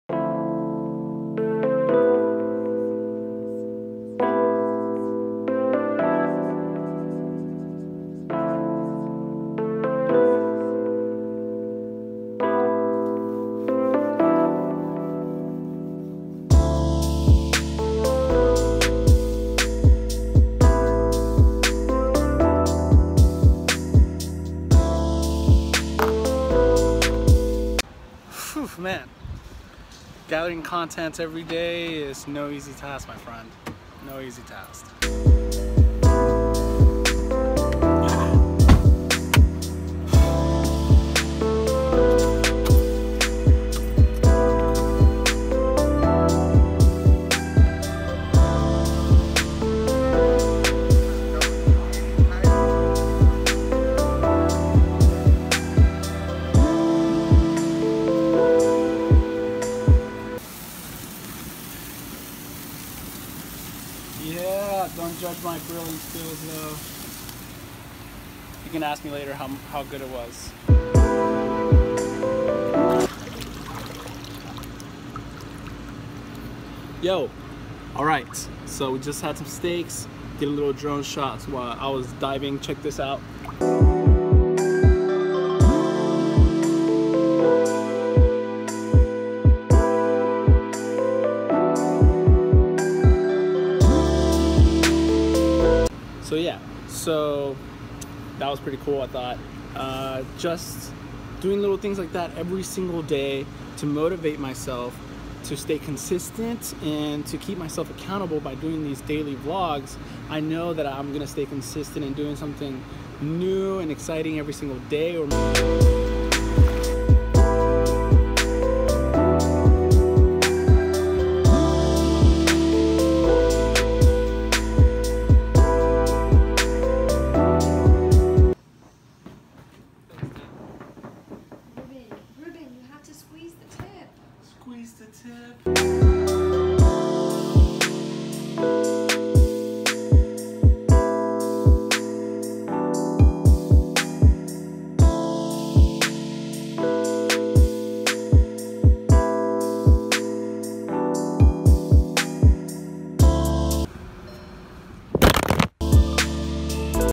Why everyone should Vlog
Vlog entry 02 Why everyone should Vlog.

Vlog numero 2. Today I take some shots and define the reasons behind creating a daily Vlog. Today I had a lot of work so I didn't want to bore you guys with that.